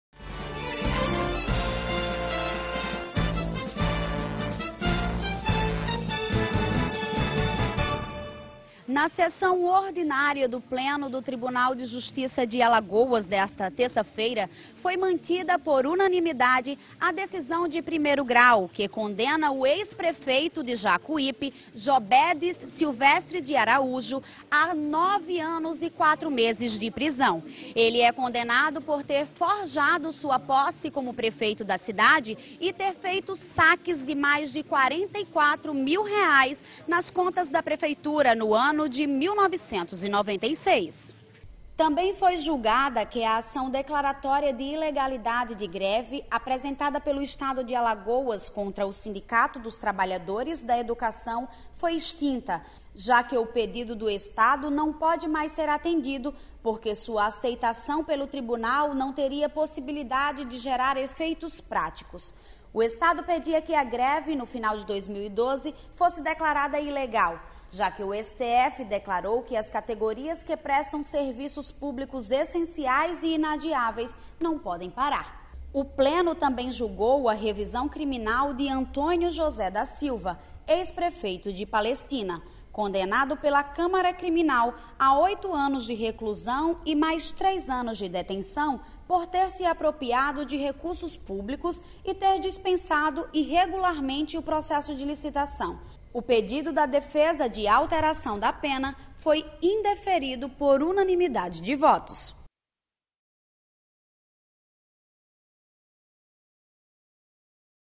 Pleno manteve pena de ex-vereador de Jacuípe acusado de peculato
Processo do Estado contra greve da educação é extinto na mesma sessão